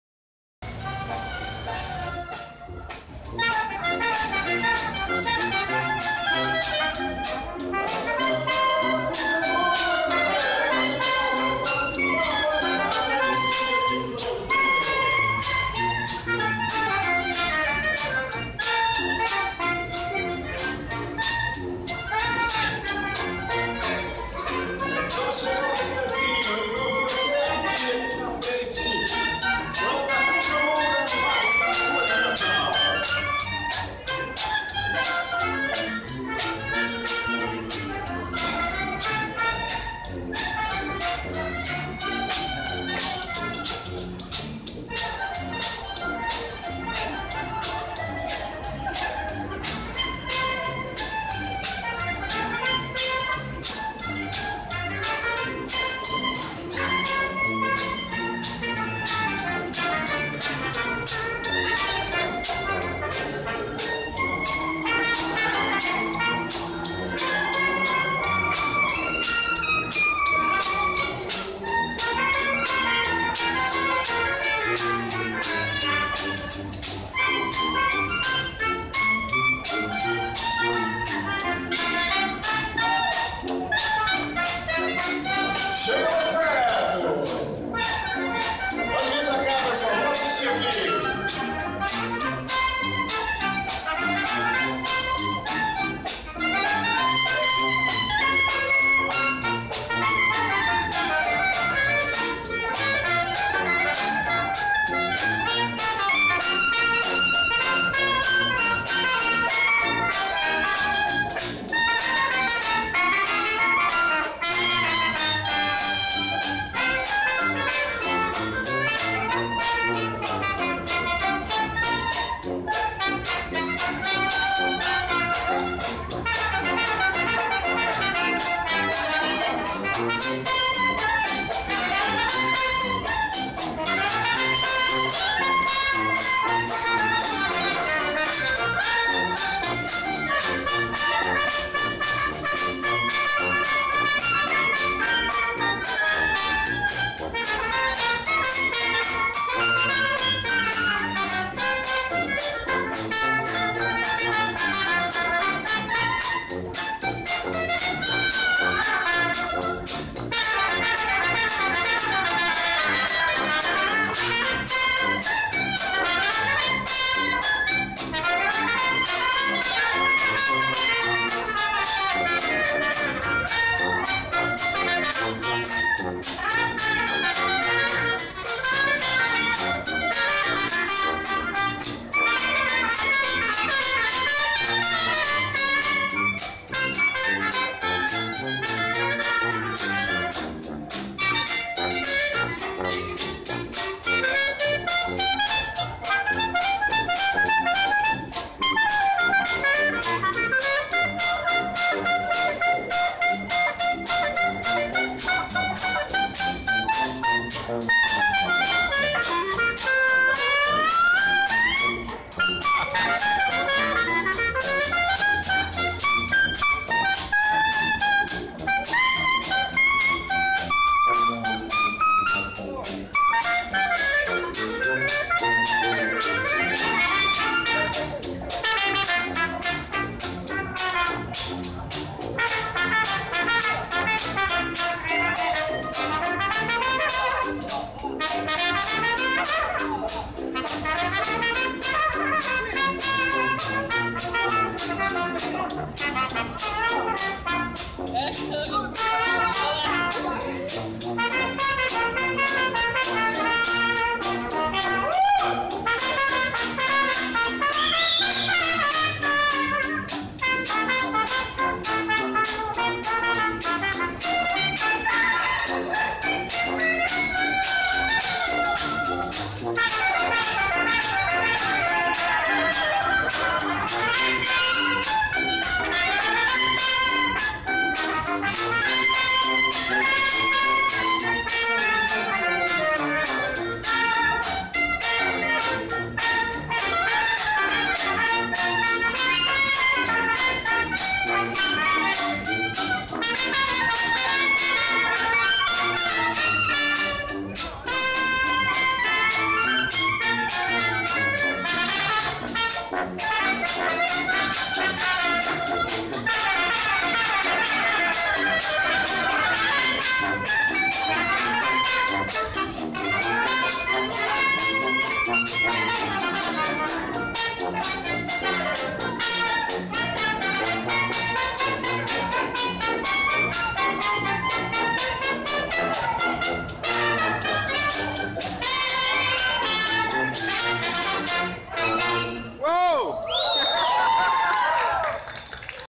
Banda Cucamonga - 02 de 07
Apresentação da Banda Cucamonga em 09 de dezembro na estação Osasco da CPTM apresentando o Dixieland (Jazz de rua)